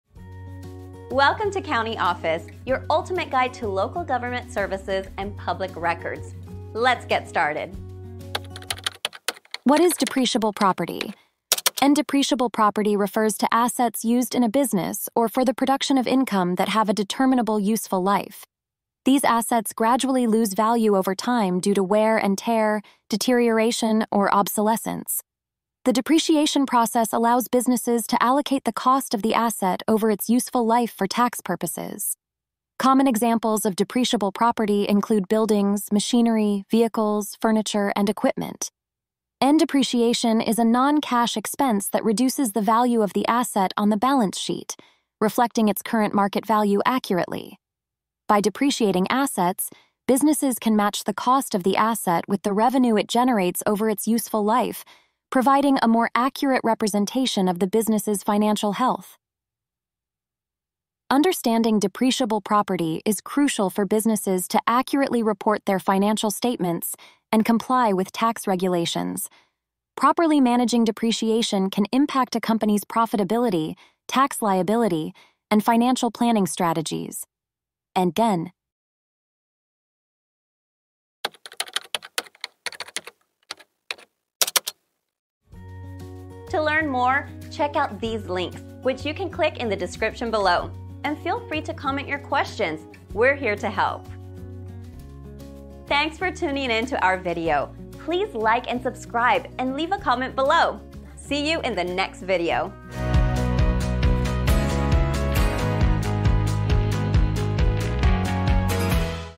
What Is Depreciable Property? - CountyOffice.org
What Is Depreciable Property? Have you ever wondered about depreciable property and its significance in business and tax matters? In this insightful video, we delve into the concept of depreciable property, its role in financial reporting, and tax implications. Through depreciation, businesses can allocate asset costs over its useful life for tax purposes, aiding in accurate financial representation.

Understanding depreciable property is fundamental for businesses to ensure accurate financial reporting and tax compliance. Properly managing depreciation can influence a company's profitability, tax obligations, and financial planning strategies significantly. Join us for a detailed discussion on depreciable property and its impact on business operations.